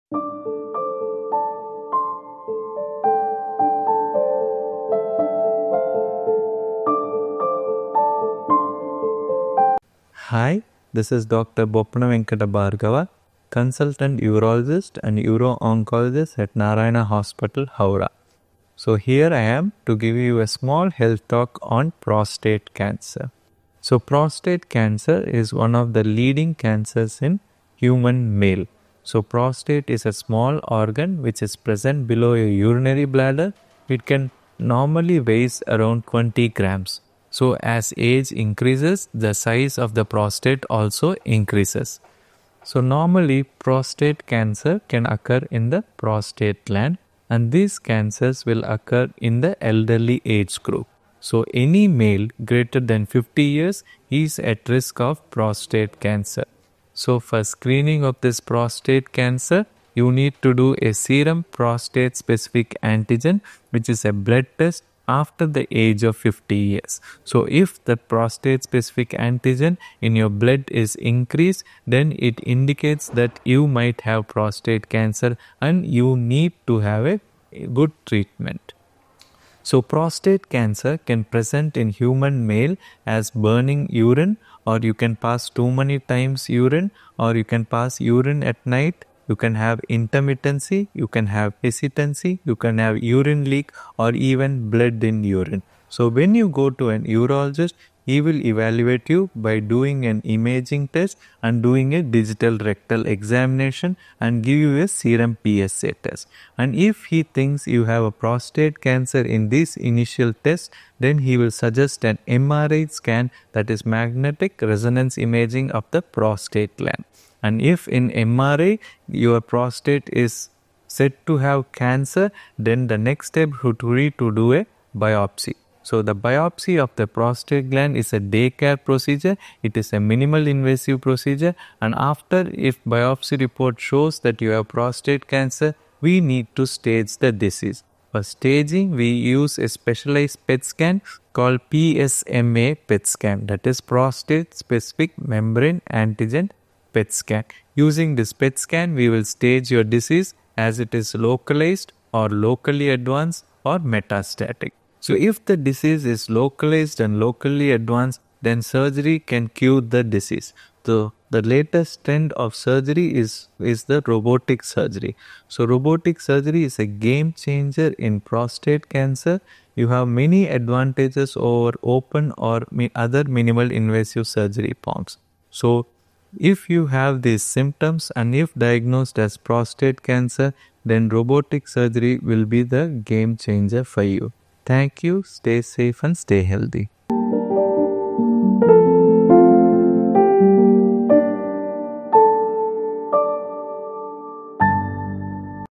Prostate Cancer Awareness | Dr. Boppana Venkata Bhargava
Prostate cancer is one of the most common cancers affecting men, particularly those over the age of 50. In this health talk, Dr. Boppana Venkata Bhargava, Consultant Urologist and Uro-oncologist at Narayana Hospital, Howrah, explains everything you need to know about prostate cancer—from symptoms and diagnosis to the latest advancements in treatment.

Key Highlights:
What is the Prostate?
The prostate is a small gland located below the bladder, typically weighing around 20 grams. Its size increases with age, and men over 50 are at higher risk of developing prostate cancer.

Symptoms of Prostate Cancer:

Burning sensation while urinating.
Frequent urination, especially at night.
Intermittent or hesitant urination.
Urine leakage or presence of blood in urine.
Diagnosis:

Screening Tests: The Prostate-Specific Antigen (PSA) blood test is recommended after the age of 50. Elevated PSA levels can indicate prostate cancer.
Imaging & Biopsy: After an initial evaluation, an MRI scan may be done to detect cancer. A prostate biopsy, a minimally invasive day-care procedure, confirms the diagnosis.
Staging:
The PSMA PET scan helps determine whether the cancer is localized, locally advanced, or metastatic.

Treatment Options:

Localized and Locally Advanced Cases: Robotic surgery offers precision and faster recovery, making it a game-changer in prostate cancer treatment.
For advanced stages, other treatment options may include radiation therapy or systemic therapies.
Why Robotic Surgery?
Robotic surgery provides numerous advantages over traditional open or minimally invasive techniques, ensuring a safer and more effective treatment for prostate cancer patients.

If you or a loved one are experiencing symptoms of prostate cancer or have elevated PSA levels, consult a urologist promptly. Early detection can save lives!